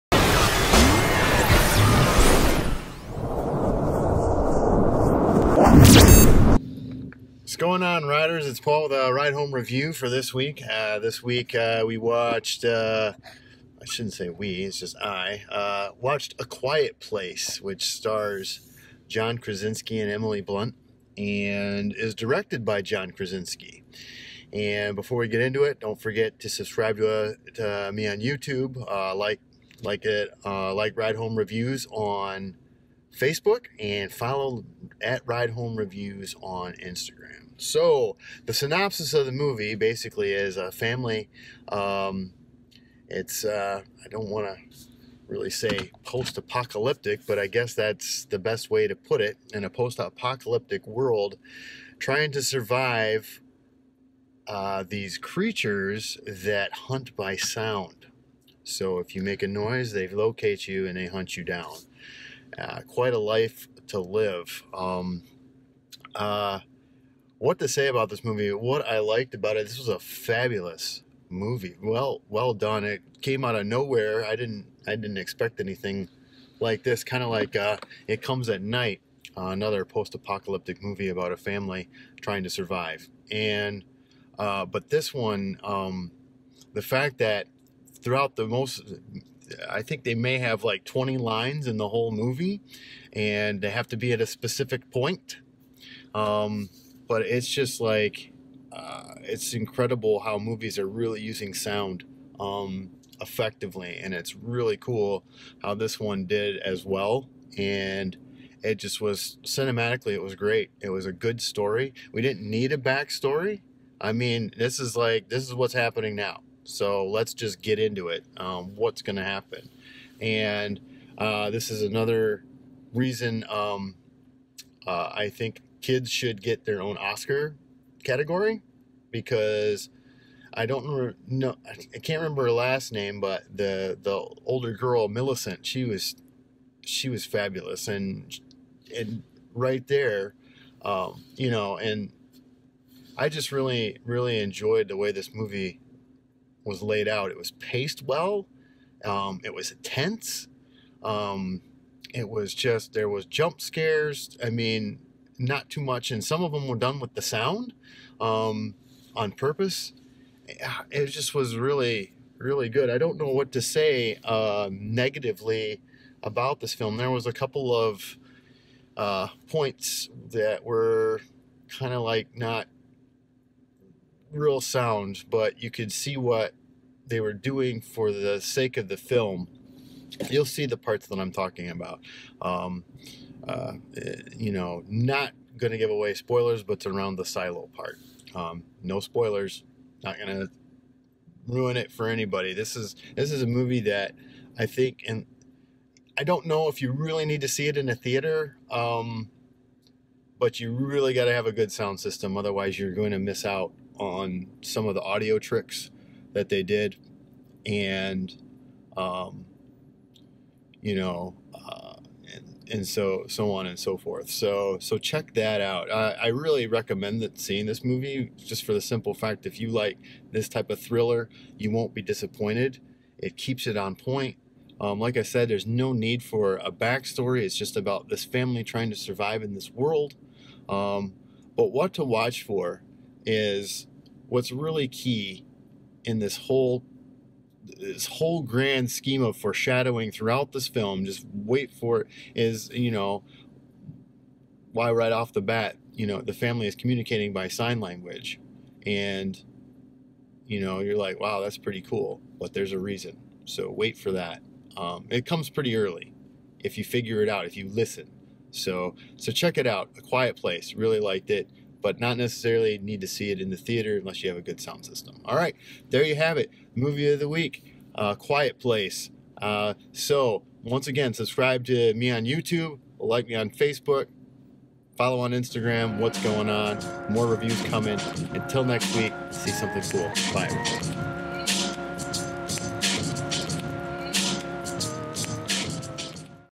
RideHomeReview - A Quiet Place
Synopsis: A family is forced to live in silence while hiding from creatures that hunt by sound.

Director: John Krasinski
Writers: Bryan Woods (screenplay by), Scott Beck (screenplay by)
Stars: Emily Blunt, John Krasinski, Millicent Simmonds

What I liked: The use of sound and lack thereof, simple storyline
What I didn't like: A few plot holes near the end.
What to watch for: The foreshadowing elements.